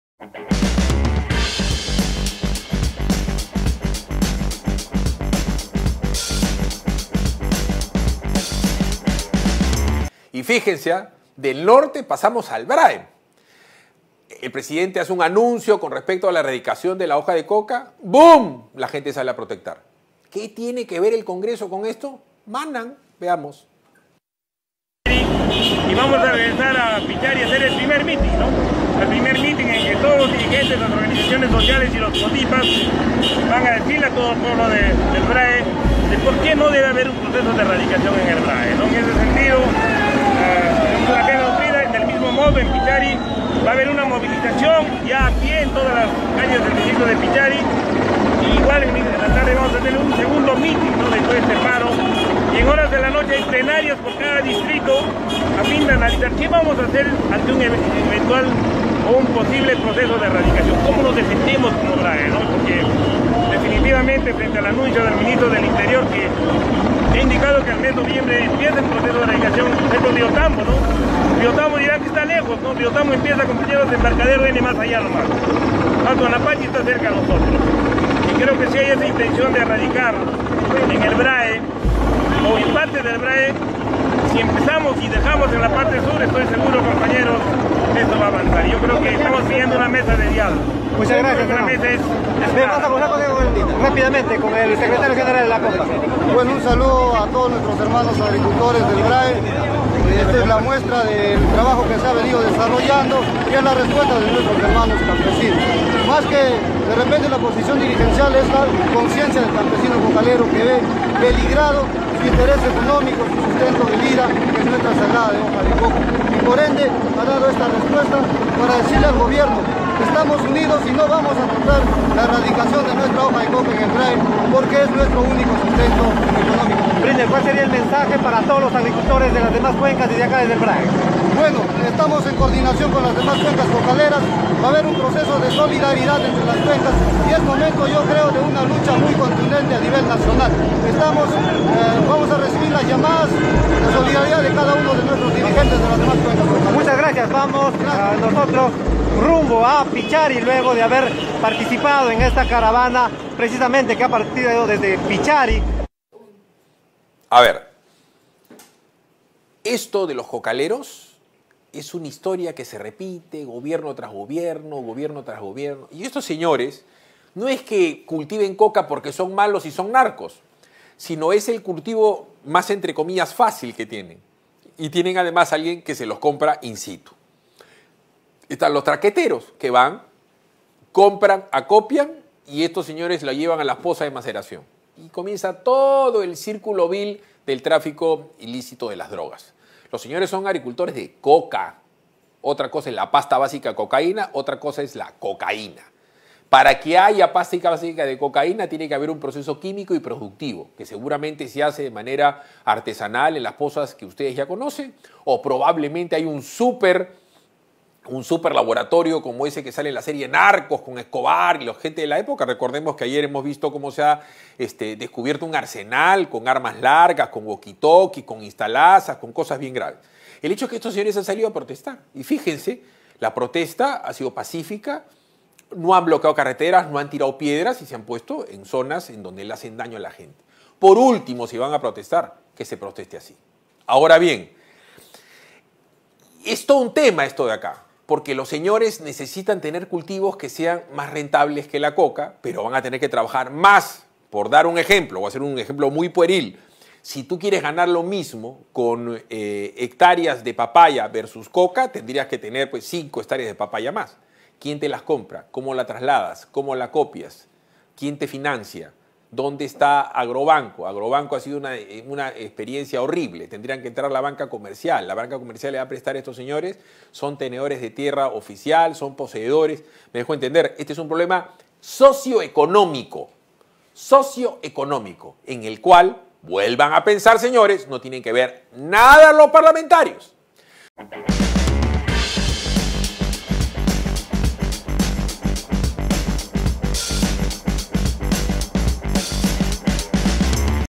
PolicialesPBO 🚓 ¡PARO COCALERO EN EL VRAEM!
¡PARO COCALERO EN EL VRAEM!
🔴 Personas protestan por anuncio de erradicación de la hoja de coca